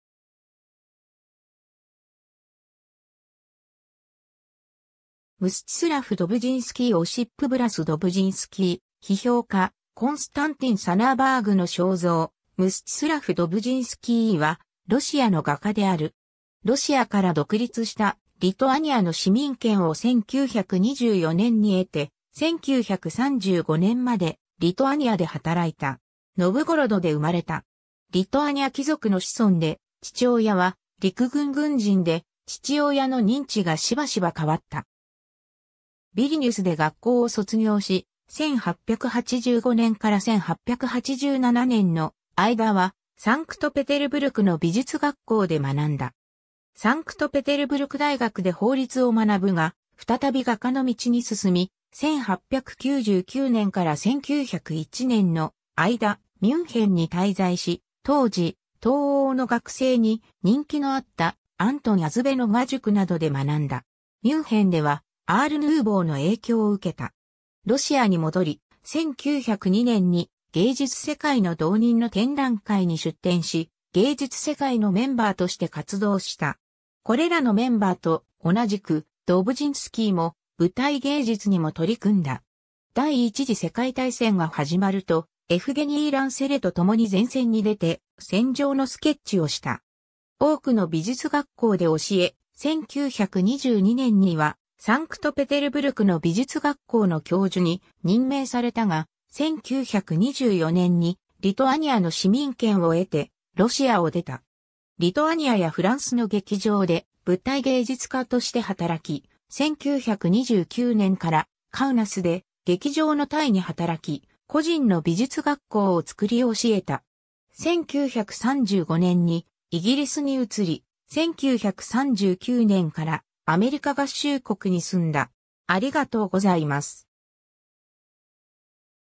ムスチスラフ・ドブジンスキー
ムスチスラフ・ドブジンスキー (画)オシップ・ブラス ドブジンスキー（作）『批評家Konstantin Sunnerbergの肖像』(1905) ムスチスラフ・ドブジンスキー（ロシア語: Мстислав Валерианович Добужинский, ラテン文字転写: Mstislav Walerianowitsch Dobuschinski、1875年8月2日 - 1957年11月20日）はロシアの画家である。
ロシアから独立したリトアニアの市民権を1924年に得て、1935年までリトアニアで働いた。
 ノヴゴロドで生まれた。
リトアニア貴族の子孫で、父親は陸軍軍人で、父親の任地がしばしば変わった。
ビリニュスで学校を卒業し、1885年から1887年の間は、サンクトペテルブルクの美術学校で学んだ。
サンクトペテルブルク大学で法律を学ぶが、再び画家の道に進み、1899年から1901年の間、ミュンヘンに滞在し、当時、東欧の学生に人気のあった、アントン・アズベ(Anton Ažbe)の画塾などで学んだ。
ミュンヘンではアール･ヌーボーの影響を受けた。
 ロシアに戻り、1902年に、『芸術世界』（ミール・イスクーストヴァ）の同人の展覧会に出展し、『芸術世界』のメンバーとして活動した。
これらのメンバーと同じくドブジンスキーも舞台芸術にも取り組んだ。
 第一次世界大戦が始まるとエフゲニー・ランセレとともに前線に出て戦場のスケッチをした。
多くの美術学校で教え、1922年には、サンクトペテルブルクの美術学校の教授に任命されたが、1924年にリトアニアの市民権を得てロシアを出た。
 リトアニアやフランスの劇場で舞台芸術化として働き、1929年からカウナスで、劇場のたえに働き、個人の美術学校を作り教えた。
1935年にイギリスに移り、1939年からアメリカ合衆国に住んだ。